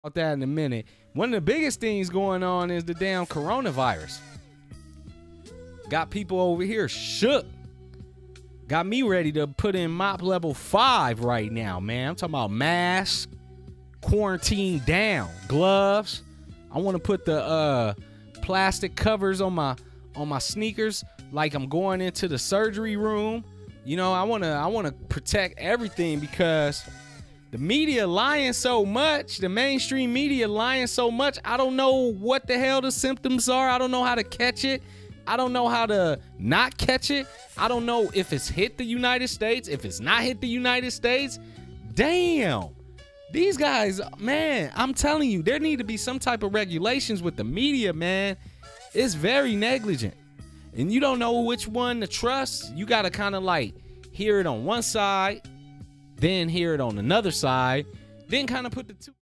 Corona Virus Has Forced Every Casino On The Las Vegas Strip To Close | FCP Sports Radio (Preview)
S3.Ep22.  The Corona Virus has the whole city of Las Vegas on lock down including the strip.  R'Dot and the FCP Sports Radio Show rant about the impact the media has made with the around-the-clock coverage of the pandemic.  (Video shows an empty Las Vegas Strip)

For full episodes of the FCP Sports Radio show, follow